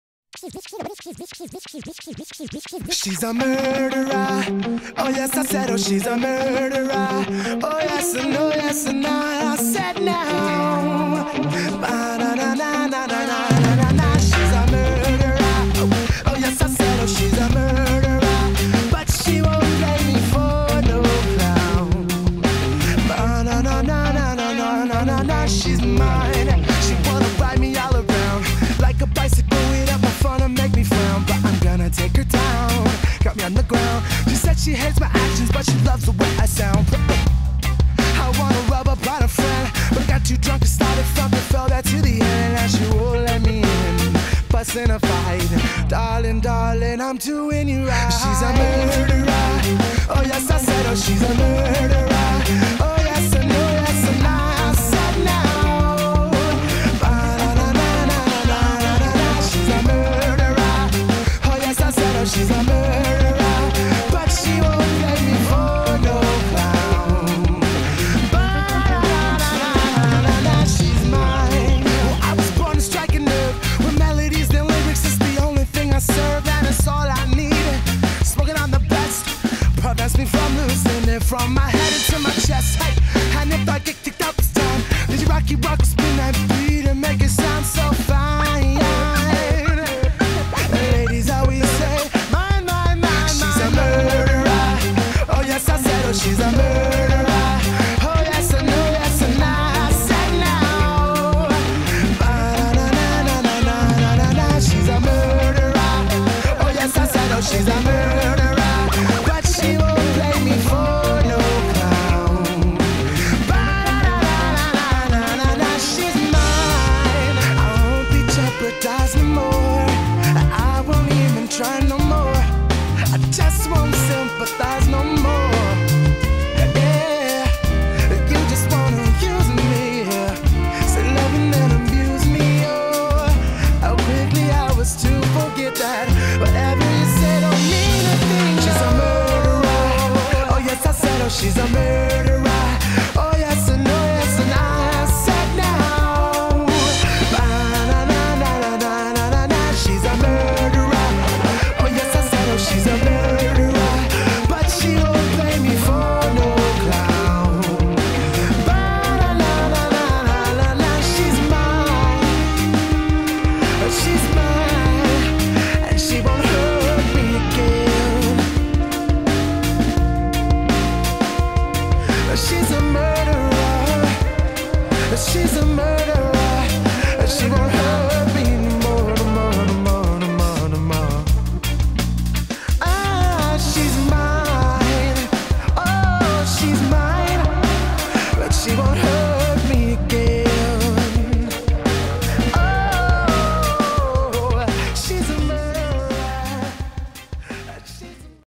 Basic aerial combo {Balisong}
Basic Aerial combo  A no nonsense tutorial without all the talking. Straight to the point! Your once stop shop for Balisong tricks.